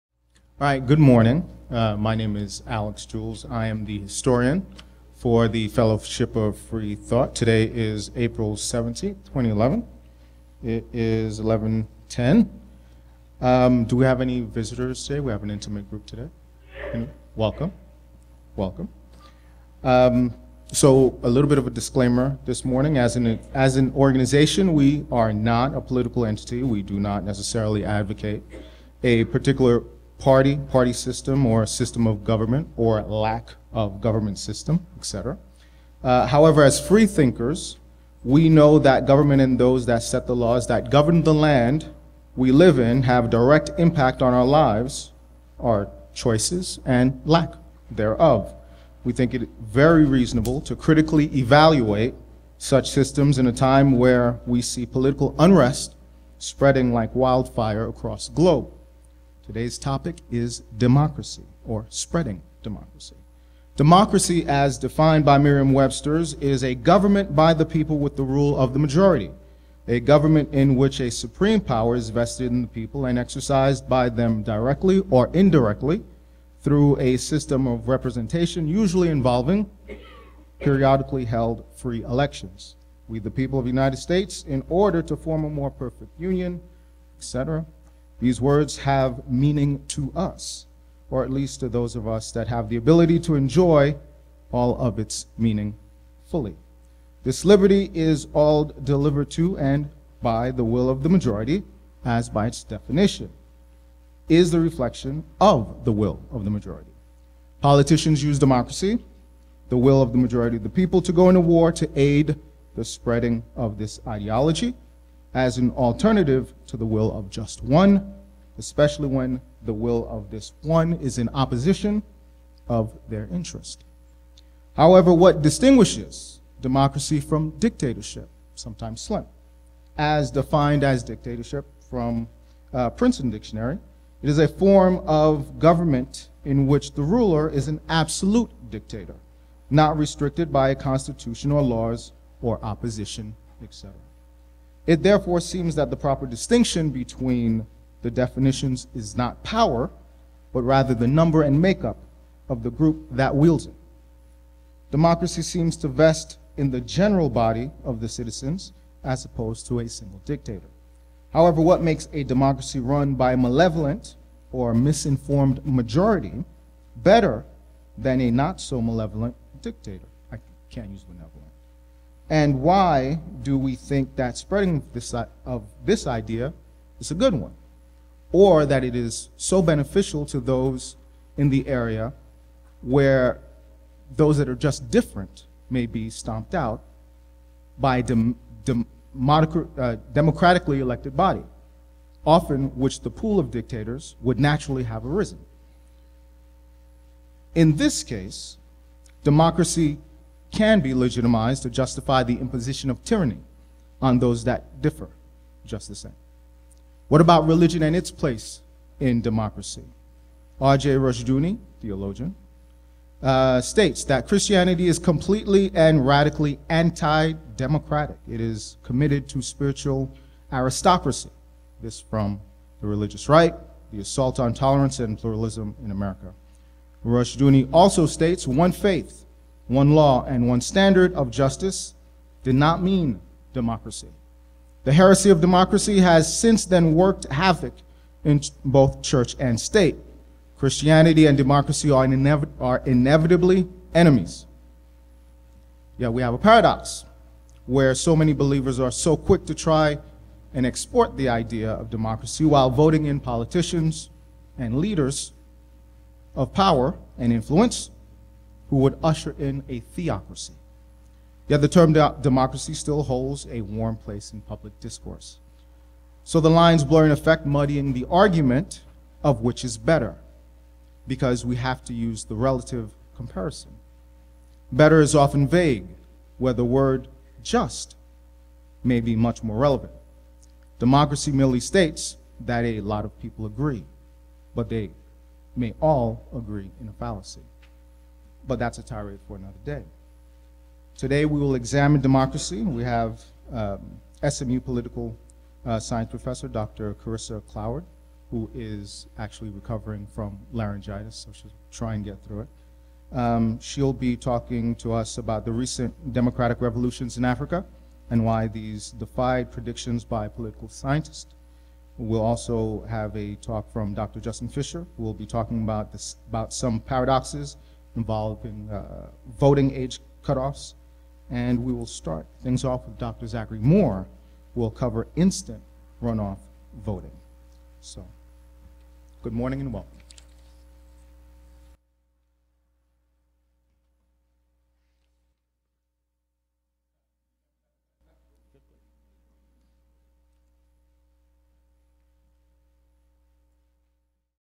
Welcome & Introduction — "Spreading Democracy"
Gathering — April 2011 — "Spreading Democracy"

Alix Jules welcomes everyone, and introduces the topic of the day:  "Spreading Democracy".